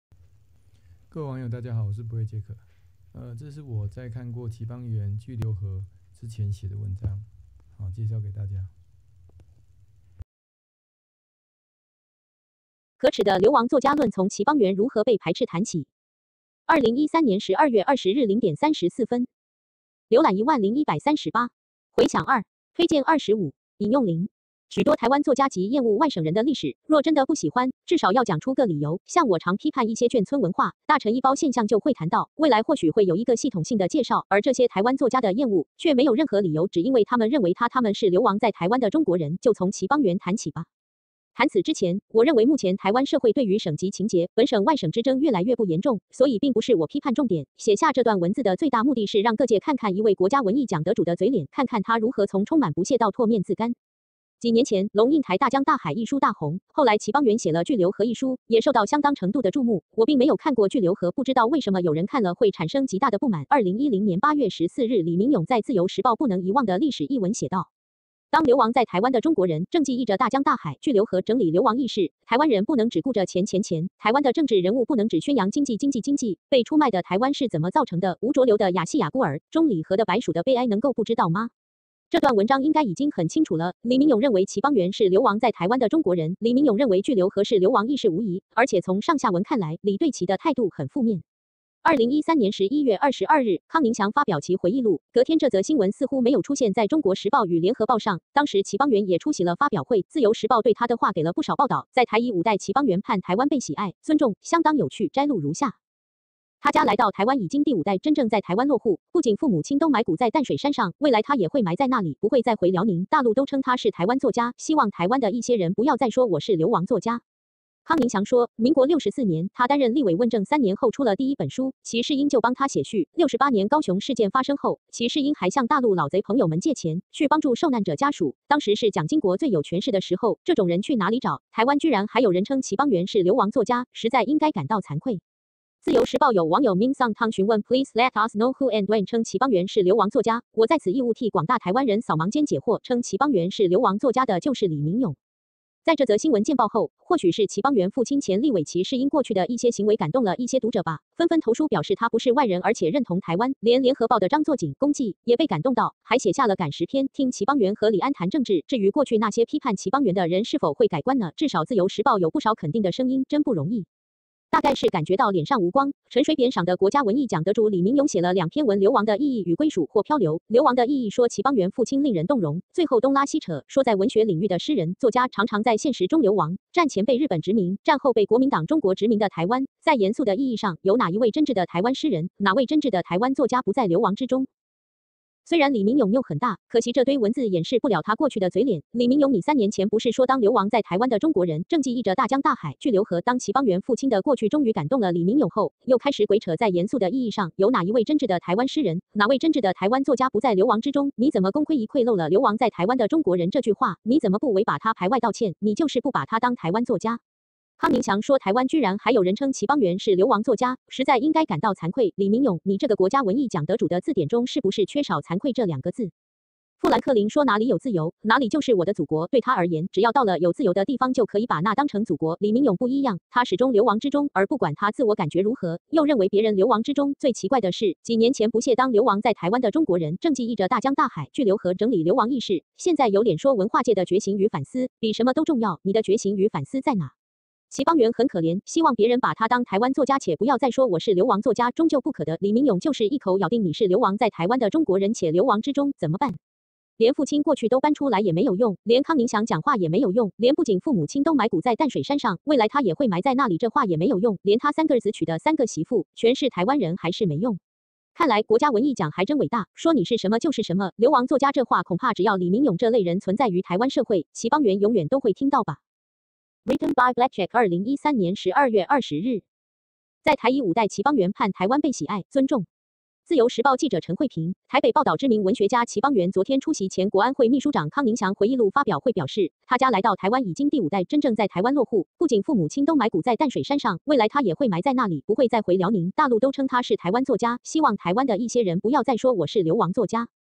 可恥的「流亡作家」論－從齊邦媛《巨流河》如何被排斥談起
可恥的「流亡作家」論－從齊邦媛如何被排斥談起
許多台灣作家極厭惡外省人的「歷史」，若真的不喜歡，至少要講出個理由，像我常批判一些眷村文化、大陳義胞現象就會談到（未來或許會有一個系統性的介紹）。而這些台灣作家的「厭惡」，卻沒有任何理由，只因為他們認為她他們是「流亡在台灣的中國人」，就從齊邦媛談起吧。

談此之前，我認為目前台灣社會對於省籍情結、本省外省之爭越來越不嚴重，所以並不是我批判重點。寫下這段文字的最大目的是讓各界看看一位國家文藝獎得主的嘴臉，看看他如何從充滿不屑到唾面自乾。

幾年前，龍應台《大江大海》一書大紅，後來齊邦媛寫了《巨流河》一書，也受到相當程度的注目，我並沒有看過《巨流河》，不知道為什麼有人看了會產生極大的不滿。2010/08/14李敏勇在自由時報不能遺忘的歷史一文寫道：

…當流亡在台灣的中國人，正記憶著《大江大海》、《巨流河》…整理流亡意識，台灣人不能只顧著錢錢錢，台灣的政治人物不能只宣揚經濟經濟經濟。《被出賣的台灣》是怎麼造成的？吳濁流的《亞細亞孤兒》、鍾理和的《白薯的悲哀》能夠不知道嗎？…

這段文章應該已經很清楚了，李敏勇認為齊邦媛是「流亡在台灣的中國人」，李敏勇認為《巨流河》是「流亡意識」無疑，而且從上下文看來，李對齊的態度很負面。

2013/11/22，康寧祥發表其回憶錄，隔天這則新聞似乎沒有出現在中國時報與聯合報上，當時齊邦媛也出席了發表會，自由時報對她的話給了不少報導（在台已五代 齊邦媛盼台灣被喜愛、尊重），相當有趣，摘錄如下：

她家來到台灣已經第五代，真正在台灣落戶，不僅父母親都埋骨在淡水山上，未來她也會埋在那裡，不會再回遼寧；大陸都稱她是台灣作家，希望台灣的一些人「不要再說我是流亡作家！」…

康寧祥說，民國六十四年，他擔任立委問政三年後出了第一本書，齊世英就幫他寫序；六十八年「高雄事件」發生後，齊世英還向「大陸老賊」朋友們借錢，去幫助受難者家屬，當時是蔣經國最有權勢的時候，這種人去哪裡找？台灣居然還有人稱齊邦媛是流亡作家，實在應該感到慚愧。

自由時報有網友Ming Sung Tung詢問「Please let us know who and when 稱齊邦媛是流亡作家?」，我在此義務替廣大台灣人掃盲兼解惑，稱齊邦媛是流亡作家的就是李敏勇。

在這則新聞見報後，或許是齊邦媛父親前立委齊世英過去的一些行為「感動」了一些讀者吧，紛紛投書表示她不是外人而且認同台灣，連聯合報的張作錦（龔濟）也被感動到，還寫下了「感時篇／聽齊邦媛和李安「談政治」」，至於過去那些批判齊邦媛的人是否會改觀呢？至少自由時報有不少肯定的聲音，真不容易。

大概是感覺到臉上無光，陳水扁賞的國家文藝獎得主李敏勇寫了兩篇文「流亡的意義」與「歸屬或漂流」，「流亡的意義」說齊邦媛父親「令人動容」，最後東拉西扯，說「在文學領域的詩人、作家，常常在現實中流亡。戰前被日本殖民，戰後被國民黨中國殖民的台灣，在嚴肅的意義上，有哪一位真摯的台灣詩人、哪位真摯的台灣作家不在流亡之中？！」

雖然李敏勇拗很大，可惜這堆文字掩飾不了他過去的嘴臉，李敏勇你三年前不是說「當流亡在台灣的中國人，正記憶著《大江大海》、《巨流河》…」？當齊邦媛父親的過去終於感動了李敏勇後，又開始鬼扯「在嚴肅的意義上，有哪一位真摯的台灣詩人、哪位真摯的台灣作家不在流亡之中？」，你怎麼「功虧一簣」漏了「流亡在台灣的中國人」這句話？你怎麼不為把她排外道歉？你就是不把她當「台灣作家」？

康寧祥說「台灣居然還有人稱齊邦媛是流亡作家，實在應該感到慚愧。」，李敏勇，你這個國家文藝獎得主的字典中是不是缺少「慚愧」這兩個字？

富蘭克林說「哪里有自由，哪里就是我的祖國」，對他而言，只要到了有自由的地方就可以把那當成「祖國」。李敏勇不一樣，他始終「流亡之中」，而不管他自我感覺如何，又認為別人「流亡之中」。最奇怪的是，幾年前不屑「當流亡在台灣的中國人，正記憶著《大江大海》、《巨流河》…整理流亡意識」，現在有臉說「文化界的覺醒與反思，比什麼都重要！」，你的覺醒與反思在哪？

齊邦媛很可憐，希望別人把她當「台灣作家」且「不要再說我是流亡作家！」終究不可得，李敏勇就是一口咬定妳是「流亡在台灣的中國人」且「流亡之中」，怎麼辦？

連父親過去都搬出來也沒有用，連康寧祥講話也沒有用，連「不僅父母親都埋骨在淡水山上，未來她也會埋在那裡」這話也沒有用，連「她三個兒子娶的三個媳婦，全是台灣人」還是沒用…

看來，國家文藝獎還真偉大，說妳是什麼就是什麼，「流亡作家」這話恐怕只要李敏勇這類人存在於台灣社會，齊邦媛永遠都會聽到吧！

Written by blackjack 2013/12/20

在台已五代 齊邦媛盼台灣被喜愛、尊重

〔自由時報記者陳慧萍／台北報導〕知名文學家齊邦媛昨天出席前國安會秘書長康寧祥回憶錄發表會表示，她家來到台灣已經第五代，真正在台灣落戶，不僅父母親都埋骨在淡水山上，未來她也會埋在那裡，不會再回遼寧；大陸都稱她是台灣作家，希望台灣的一些人「不要再說我是流亡作家！」